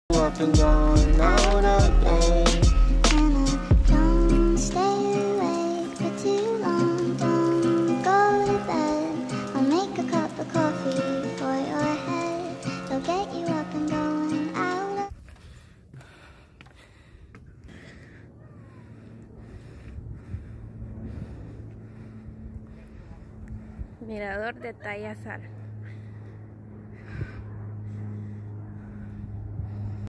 Mirador TAYASAL Petén
Hermoso mirador TAYASAL, vale la pena poder conocerlo. y la llegada es muy fácil únicamente utilizando una lancha para que los cruce de flores para el mirador.